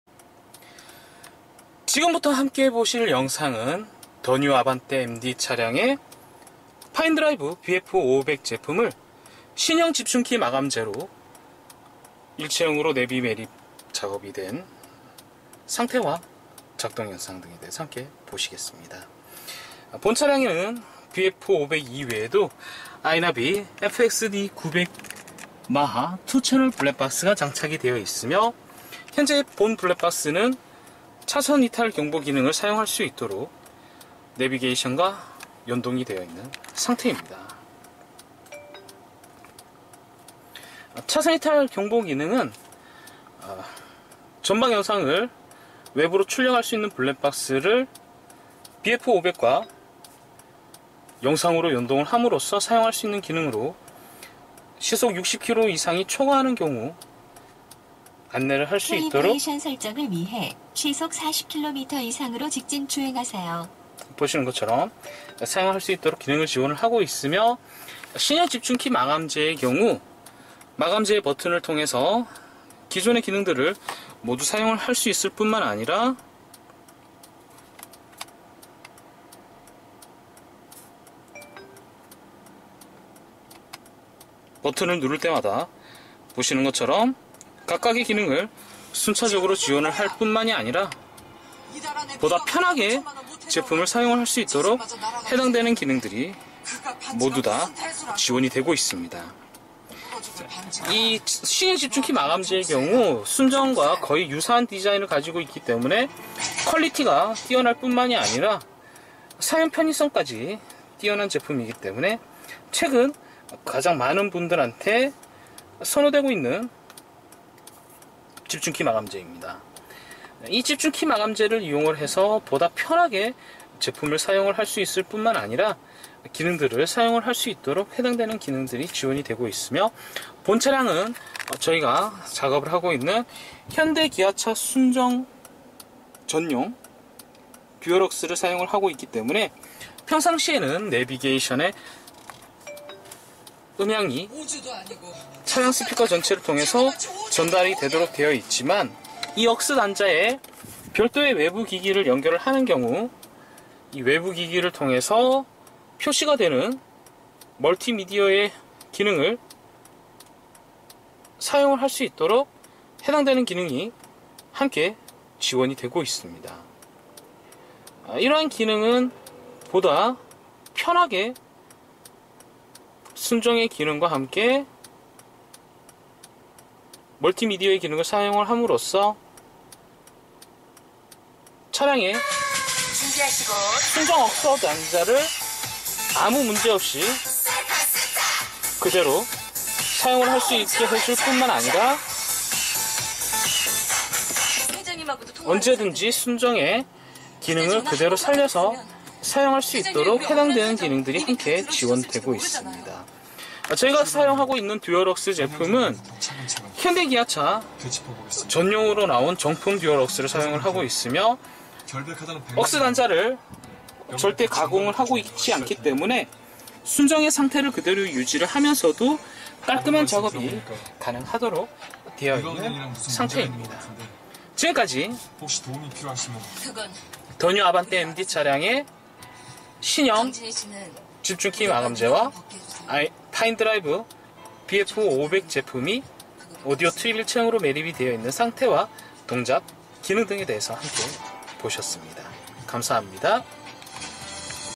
대구네비매립 더뉴아반떼MD 순정형 집중키 파인드라이브BF500 일체형 네비매립 아이오토
본 작동영상은 더뉴아반떼MD 차량에
순정형 재영집중키 마감재와 파인드라이브BF500 이
오디오 일체형으로 매립된 후의 작동영상이며,
2채널 Full HD 블랙박스인 아이나비FXD900마하 제품과
AV-in 링크를 통한 차선이탈 감지 기능을
사용할 수 있도록 작업되어 있는 상태와 동작을 함께 설명하고
있습니다.

그 이외의 추가기능은 현대기아차 전용 순정AUX 용
정품 Dual AUX를 사용하여 작동하는 상태를 함께
보여주고 있습니다.

Dual AUX의 경우 차량의 순정 AUX를 가공하지 않고
그래도 사용하면서
내비게이션의 소리를 차량 오디오로 나오게 하는
제품으로서
현대기아차 순정전용의 정품을 사용합니다.

자세한 문의는

대구 아이나비 남산점 아이오토로 문의 주시면 됩니다.